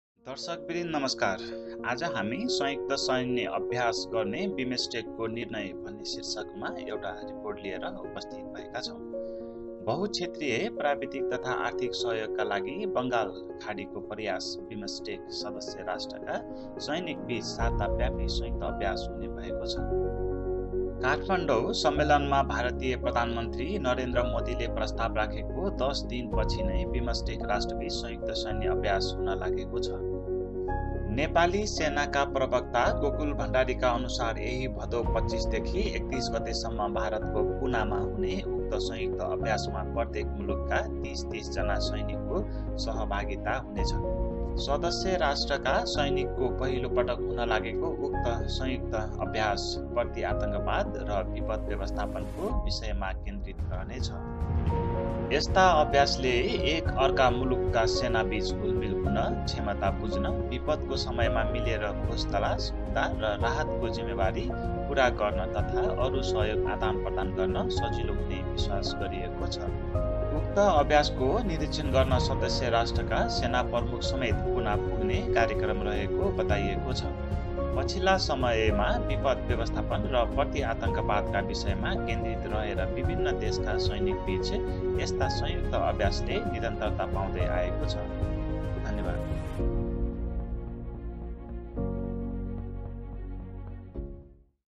संयुक्त सैन्य अभ्यास गर्ने बिमस्टेकको निर्णय
Sound  : Dhurba Giri
Camera/edit : Homenath Bhandari / Yubarna Poudel
Editor   : Dhurba Giri